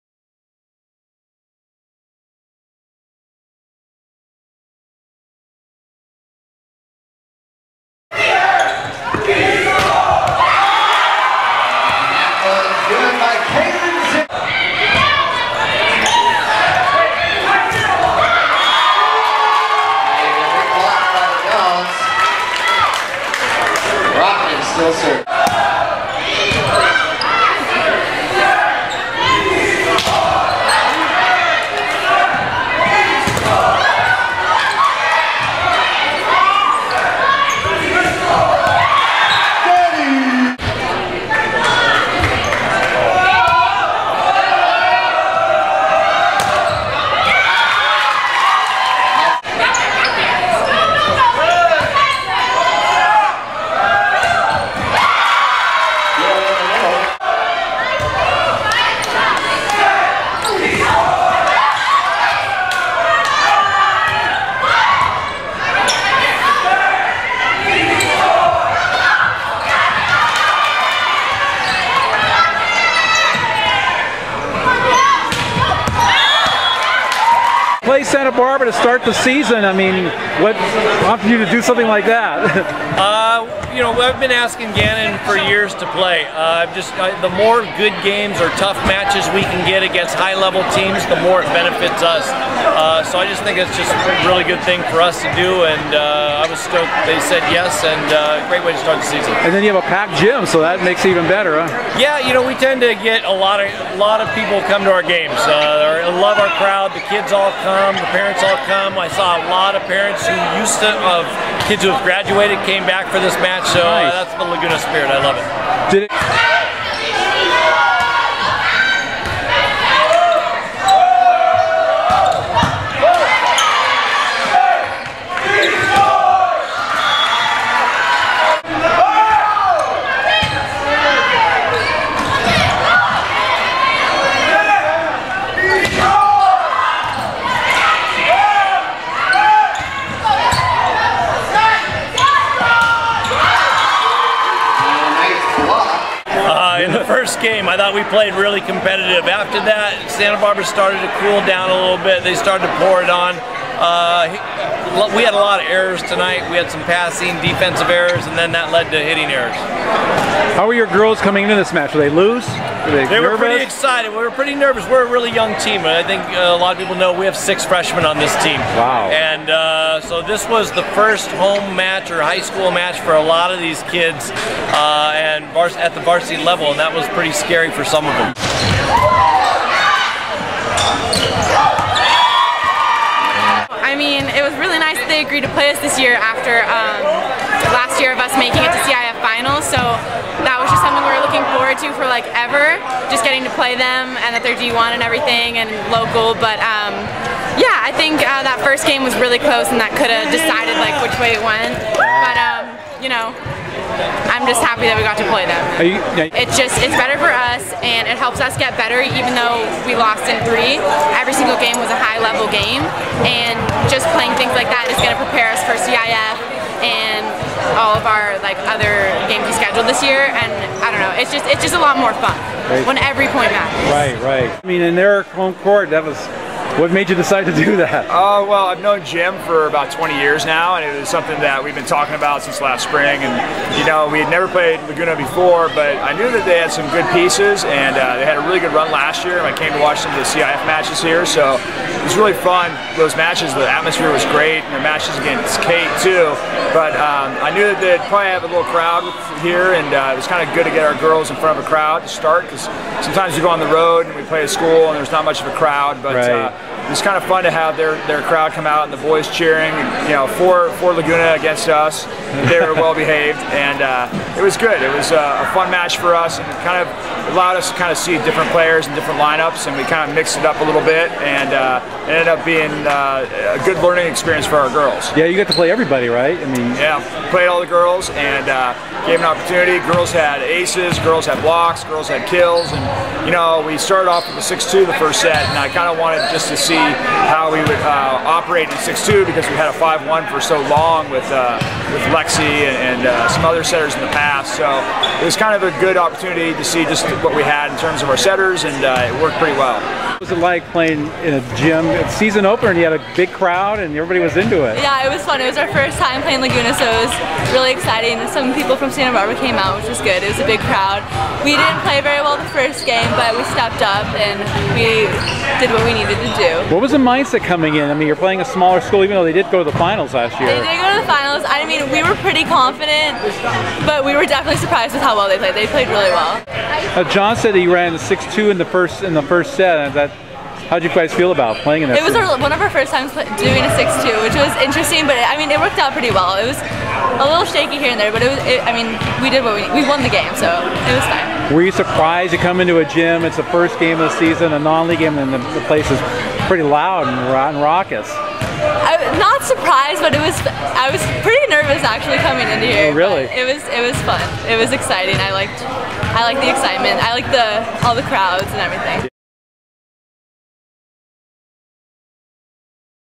Dons, Owls open girls volleyball season
Santa Barbara High sweeps Laguna Blanca in the first-ever meeting between the local girls volleyball powers